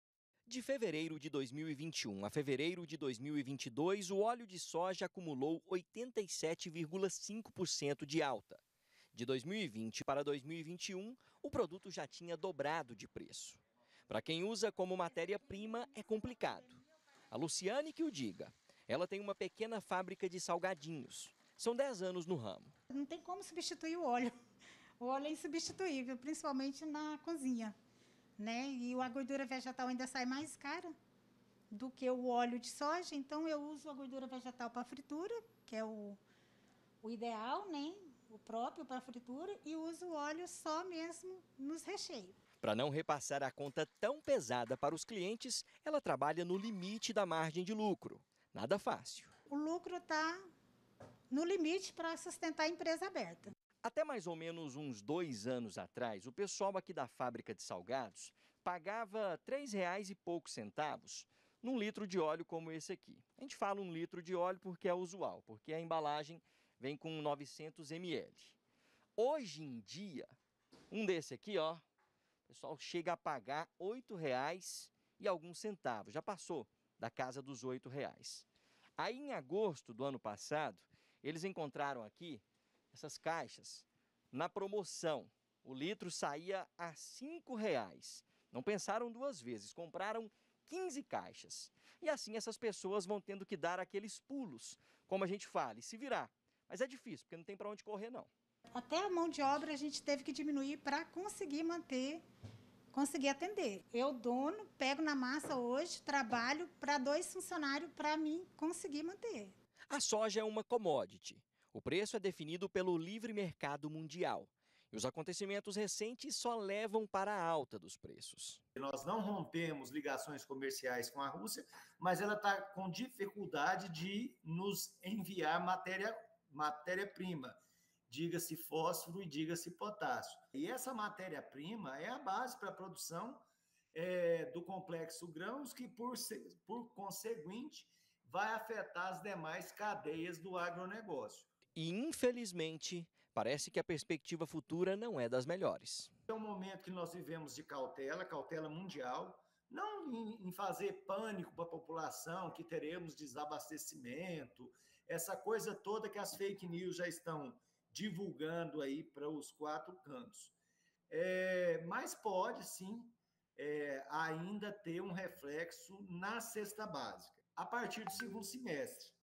FRITURA MAIS CARA: PREÇO DO ÓLEO DE COZINHA NÃO PARA DE SUBIR
DE DOIS ANOS PRA CÁ, NADA SUBIU MAIS QUE O ÓLEO DE COZINHA. O PREÇO DO LITRO PRATICAMENTE TRIPLICOU E O CONSUMIDOR SENTIU O PESO NO ORÇAMENTO.